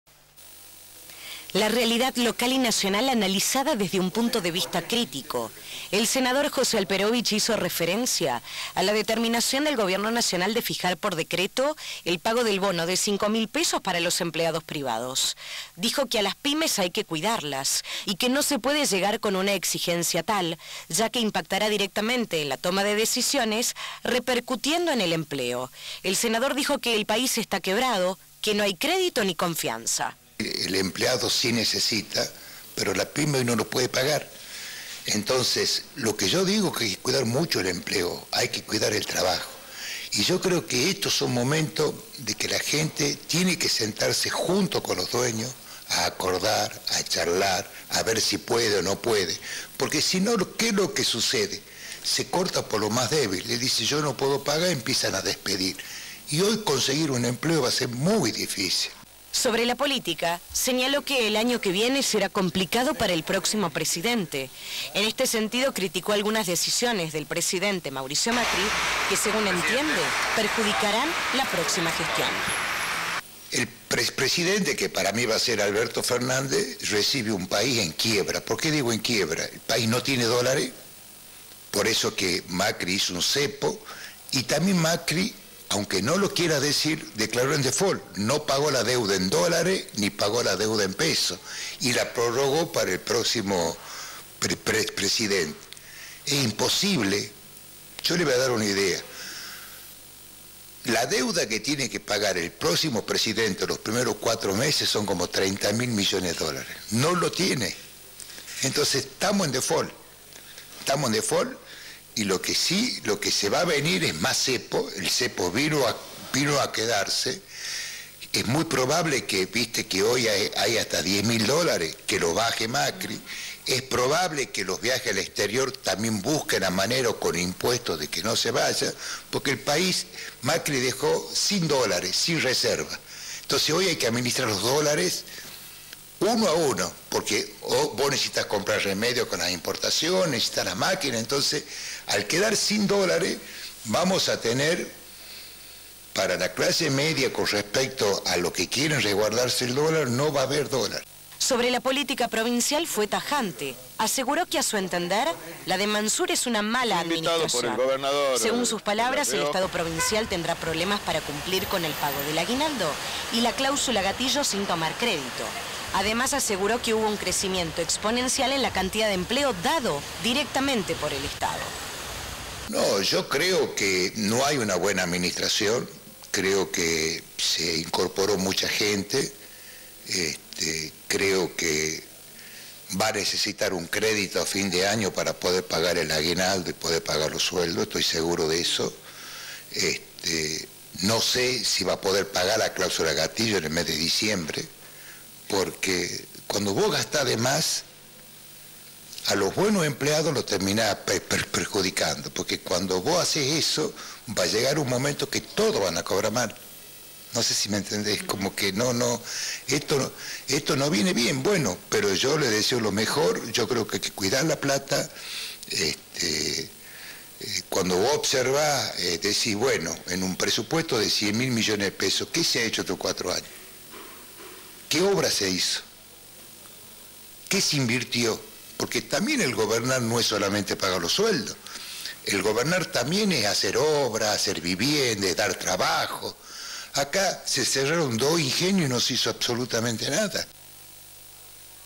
Alperovich criticó a los gobiernos local y nacional
El senador José Alperovich tiene una visión crítica en su análisis con respecto a la realidad política y económica tanto a nivel nacional como provincial.
Aseguró que Argentina está en quiebra, y cuestionó la posibilidad de la provincia de cumplir con las pautas salariales y de cláusula gatillo.